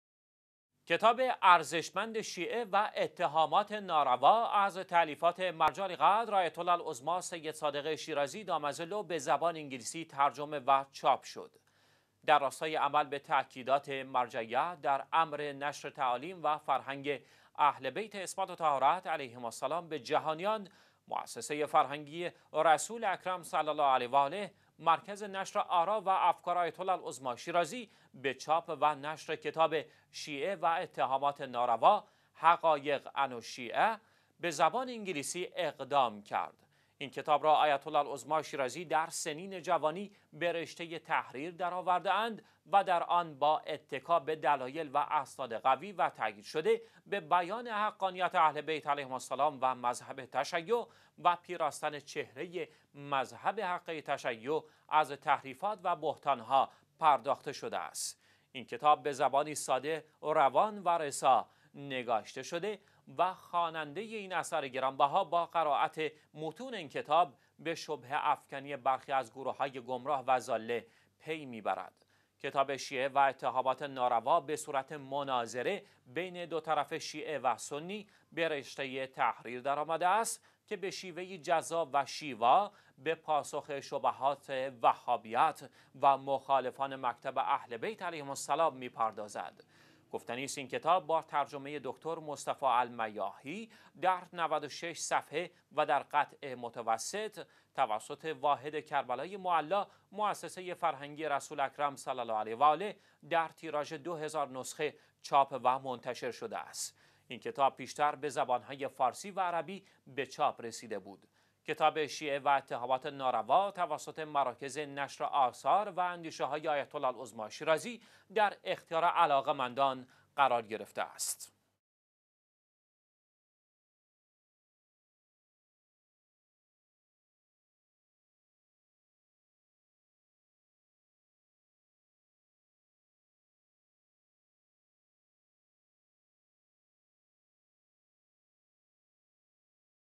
ترجمه و چاپ کتاب «شیعه و اتهامات ناروا»، به زبان انگلیسی
کتاب ارزشمند «شیعه و اتهامات ناروا»، از تألیفات مرجع عالیقدر آیت الله العظمی سید صادق حسینی شیرازی دام ظله، به زبان انگلیسی ترجمه و چاپ شد.

در راستای عمل به تاکیدات مرجع عالیقدر آیت الله العظمی سید صادق حسینی شیرازی دام ظله در امر نشر تعالیم و فرهنگ اهل_بیت عصمت و طهارت علیهم السلام به جهانیان موسسه فرهنگی رسول اکرم صلی الله علیه و آله، مرکز نشر آرا و افکار آیت_الله_العظمی_شیرازی اقدام به چاپ و نشر کتاب «شیعه و اتهامات ناروا» (حقایق عن الشیعه) به زبان انگلیسی کرد.

این کتاب را مرجع عالیقدر آیت الله العظمی سید صادق حسینی شیرازی در سنین جوانی به رشته تحریر درآورده است که در آن با اتکا به دلایل و اسناد قوی و تایید شده، که در بیان حقانیت اهل بیت علیهم السلام و مذهب تشیع و پیراستن چهره مذهب حقه تشیع از تحریفات و بهتان ها نگاشته شده است.

این کتاب به زبانی ساده، روان و رسا نگاشته شده و خواننده ی این اثر گرانبها با قرائت متون این کتاب به شبه افکنی برخی از گروهای گمراه و ضاله پی می ببرد .

کتاب «شیعه و اتهامات ناروا» به صورت مناظره بین دو طرف شیعه و سنی به رشته تحریر درآمده است که به شیوه ای شیوا و جذاب به پاسخ شبهات وهابیت و مخالفان مکتب اهل بیت علیهم السلام می پردازد.

گفتنی است این کتاب دارای 96 صفحه و در قطع متوسط و به دست دکتر مصطفی المیاحی ترجمه و توسط موسسه فرهنگی رسول اکرم صلی الله علیه و آله چاپ و منتشر شده است.

این کتاب پیش تر به زبان های فارسی و عربی به چاپ رسیده بود و اخیراً به همت مؤسسه فرهنگی ـ مذهبی رسول اکرم صلی الله علیه وآله (واحد کربلای معلی) به زبان انگلیسی ترجمه و در تیراژ 2000 عدد در قطع رقعی به چاپ رسید.

کتاب مذکور توسط مراکز نشر آثار و اندیشه های آیت الله العظمی سید صادق حسینی شیرازی دام ظله در اختیار علاقه مندان قرار گرفته  است.